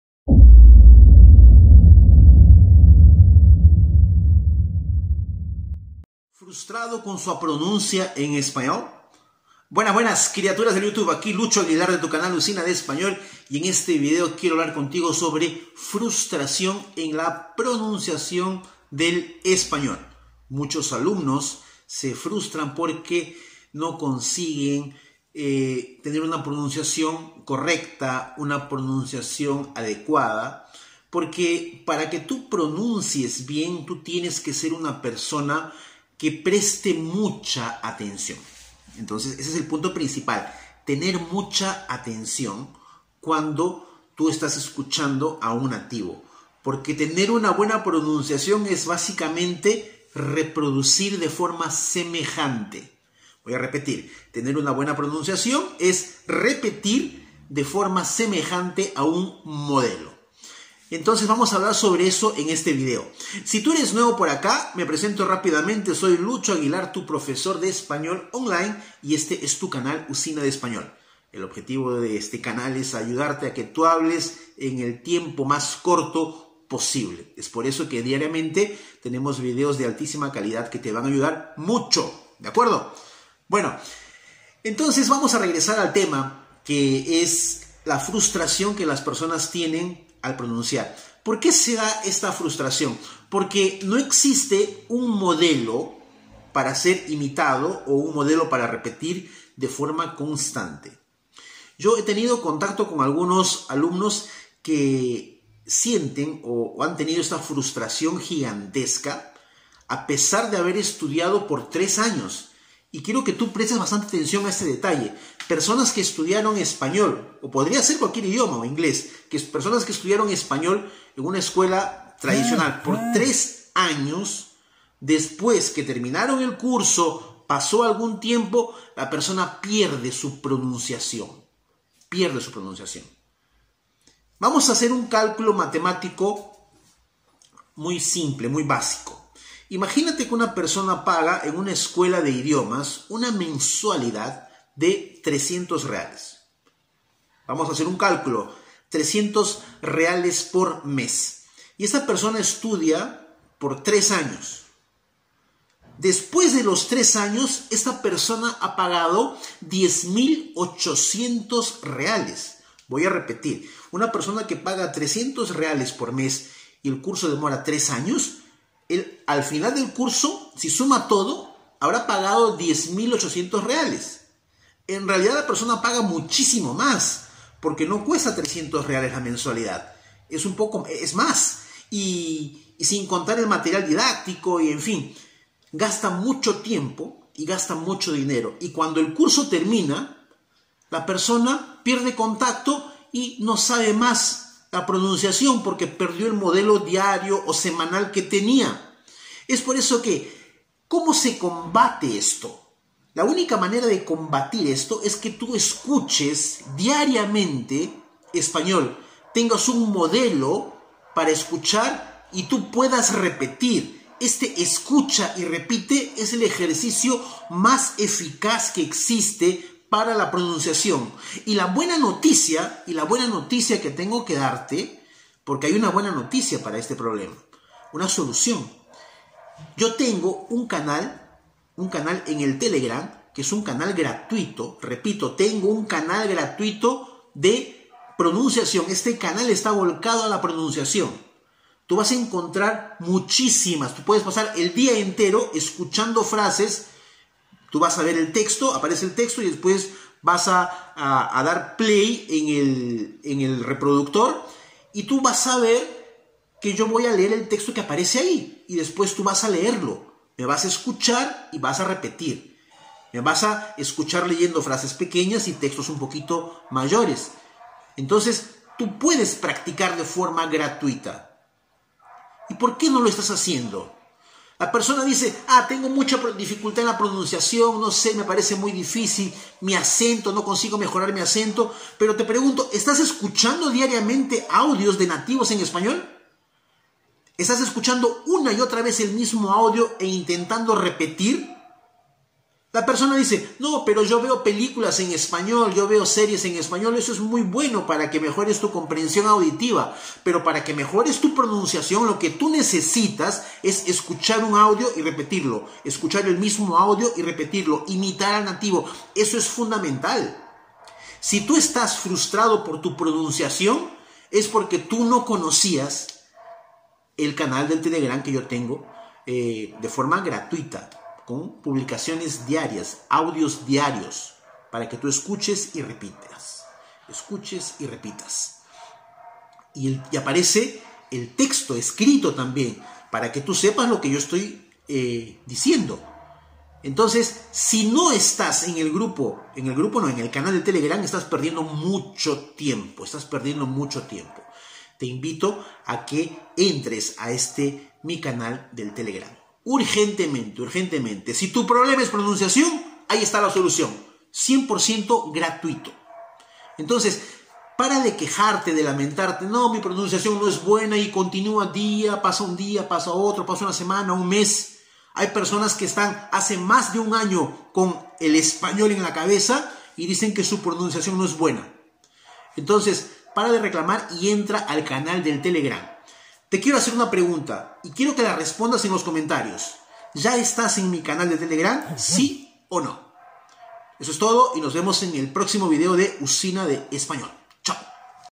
😤FRUSTRADO COM SUA PRONUNCIA EM ESPANHOL? 🇪🇸
Neste vídeo vou te mostrar como superar o obstáculo da pronúncia em espanhol.

☑ Se tiver dúvidas ou quiser ser meu aluno online, me mande um Direct no instagram: